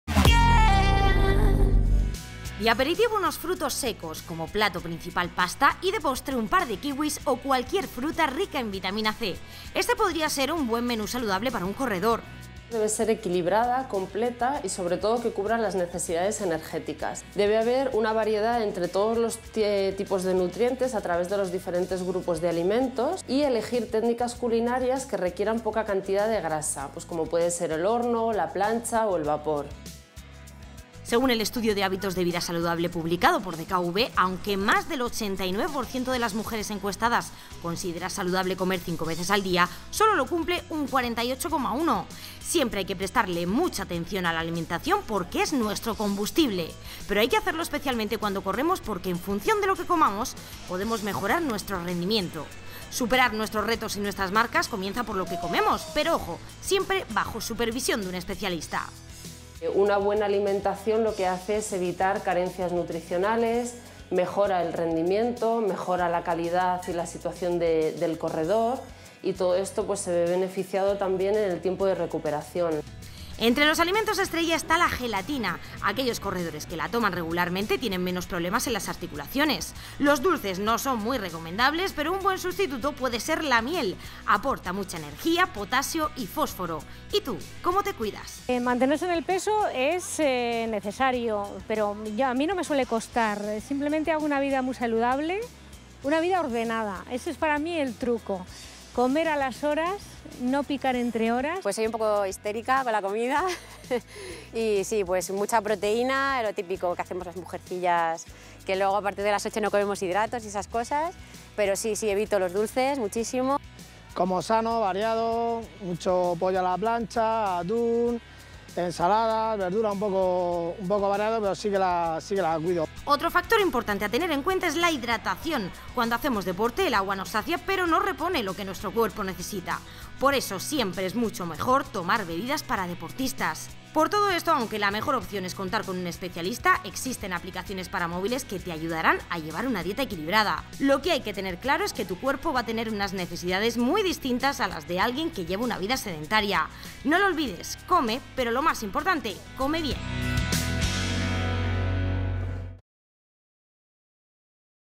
Total Running #2: Nutrición para deportistas |DKV Seguros.
¿Qué opciones son buenas para un menú saludable apto para deportistas?

Aperitivo: Frutos secos
Plato principal: Pasta
Postre: Un par de kiwis o cualquier fruta rica en vitamina C.

La dieta del corredor debe ser equilibrada, completa y que cubra las necesidades energéticas. Debe haber una variedad entre todos los tipos de nutrientes a través de los diferentes grupos de alimentos y elegir técnicas culinarias que requieran poca cantidad de grasa (horno, plancha o el vapor).

Según el I Estudio de hábitos saludables publicado por DKV, aunque más del 89% de las mujeres encuestadas considera saludable comer cinco veces al día, sólo lo cumple un 48,1%. Siempre hay que prestarle mucha atención a la alimentación porque es nuestro combustible, pero hay que hacerlo especialmente cuando corremos porque según lo comamos podemos mejorar nuestro rendimiento. Superar nuestras metas depende de nuestra alimentación, pero siempre bajo supervisión de un especialista.

Una buena alimentación lo que hace es evitar carencias nutricionales, mejora el rendimiento, la calidad del corredor y todo esto se ve beneficiado en el tiempo de recuperación.

Entre los alimentos estrella está la gelatina, aquellos corredores que la toman tienen menos problemas en las articulaciones, los dulces no son muy recomendables pero un buen sustituto puede ser la miel, nos aporta mucha energía, potasio y fósforo. Y tú, ¿cómo te cuidas?

Otro factor importante a tener en cuenta es la hidratación, cuando hacemos deporte el agua nos sacia pero no repone lo que nuestro cuerpo necesita, por lo que la mejor opción es tomar bebidas especiales para deportistas.

Aunque la mejor opción es contar con un especialista, existen APPs que nos ayudan a llevar una dieta equilibrada.

¡Come bien!